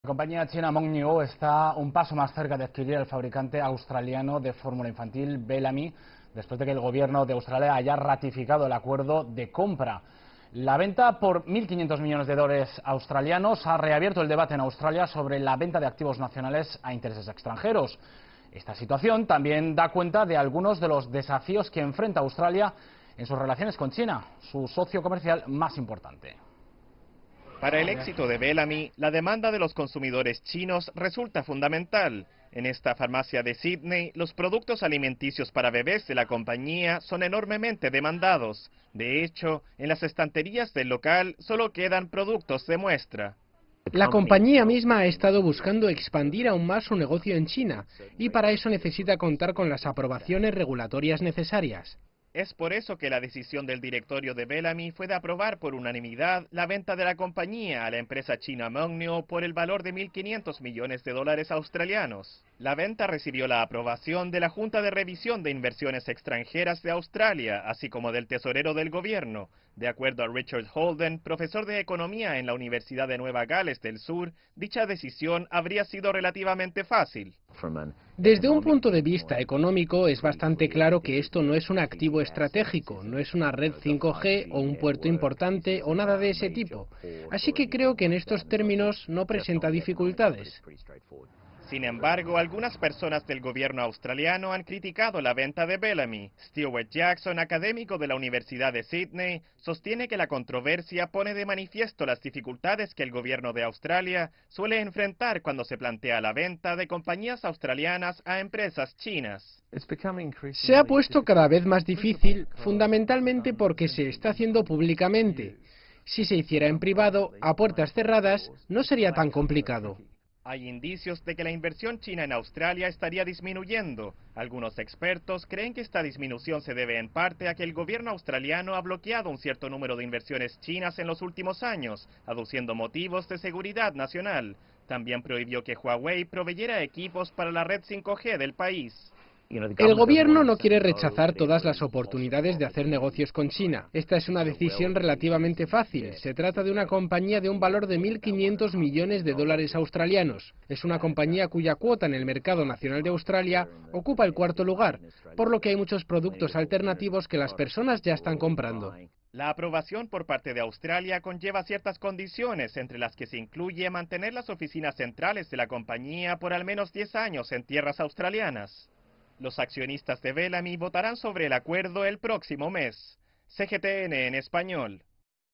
Australia aprueba la compra del fabricante de fórmula infantil Bellamy por la compañía china Mengniu
La compañía china Mengniu está un paso más cerca de adquirir al fabricante australiano de fórmula infantil, Bellamy, después de que el gobierno de Australia haya ratificado el acuerdo de compra. La venta por 1.500 millones de dólares australianos ha reabierto el debate en Australia sobre la venta de activos nacionales a intereses extranjeros. Esta situación también da cuenta de algunos de los desafíos que enfrenta Australia en sus relaciones con China, su socio comercial más importante.